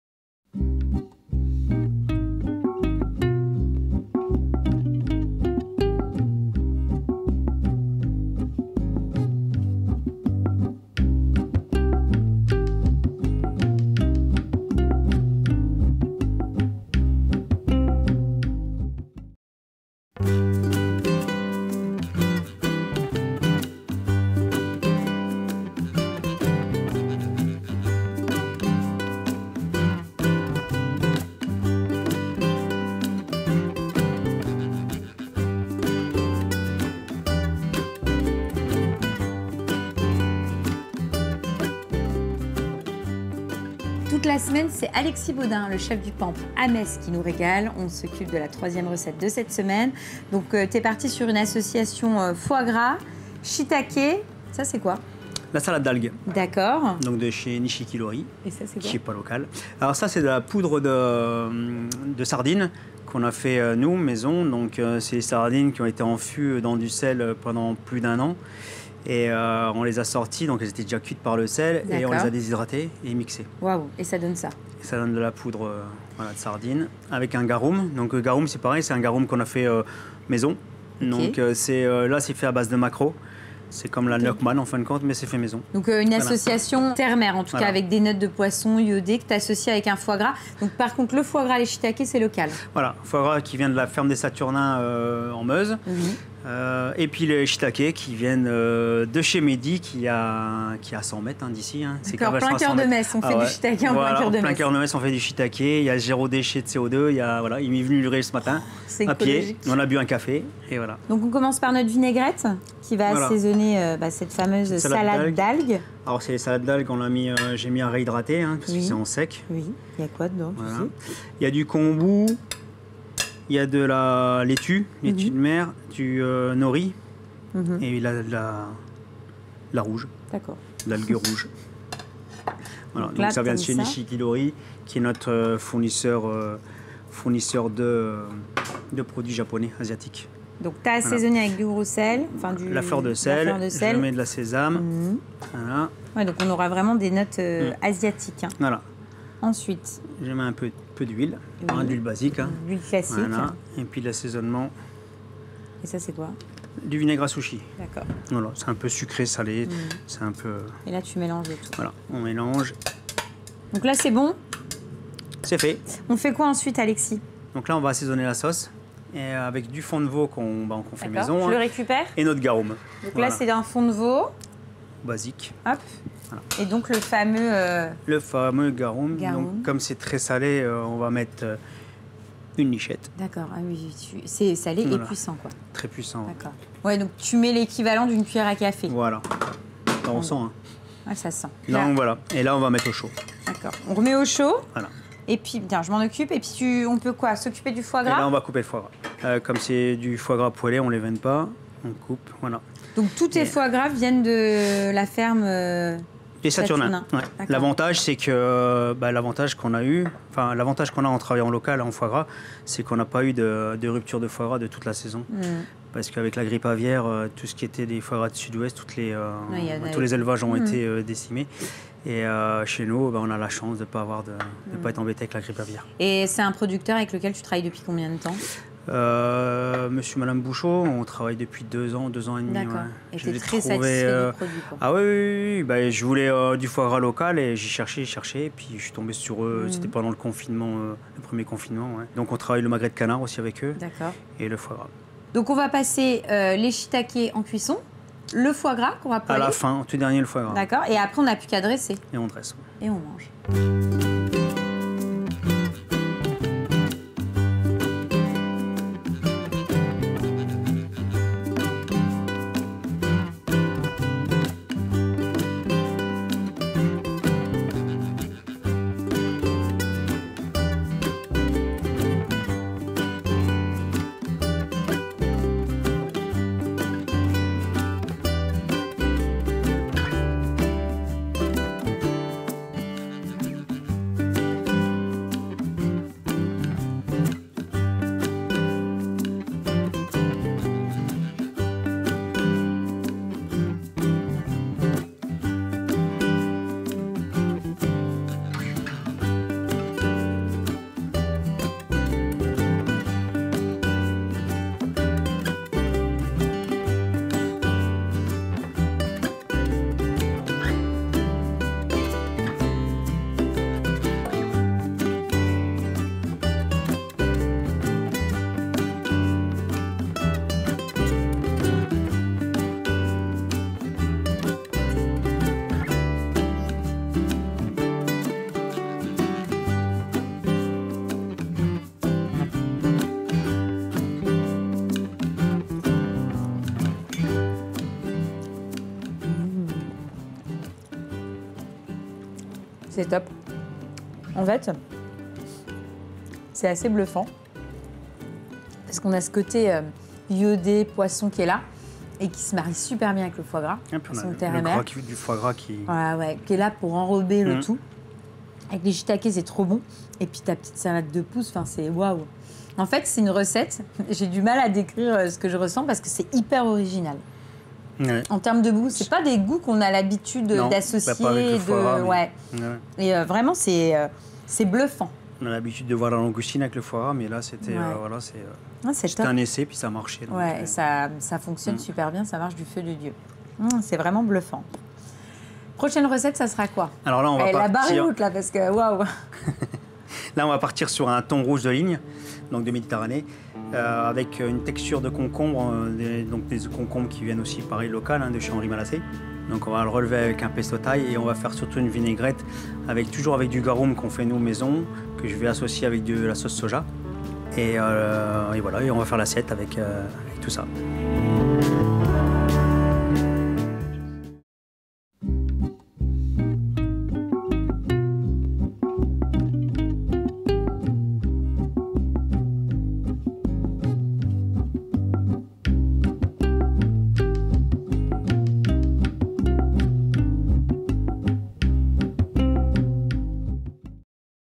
Foie gras poêlé, shitakés et salade d'algues.
À FEU DOUX avec Alexis Baudin épisode 3
Restaurant :  Le Pampre
31 Pl. de Chambre
57000 Metz
La recette :
Réaliser la salade d'algues avec votre assaisonnement préféré, émincer les shitakés et saisir à la poêle. Poêler les foies gras, servir avec un jus de veau réduit parfumée au garum.
00:00 - generique
00:20 - intro
00:44 - recette
05:02 - dressage
05:35 - degustation
06:54 - generique fin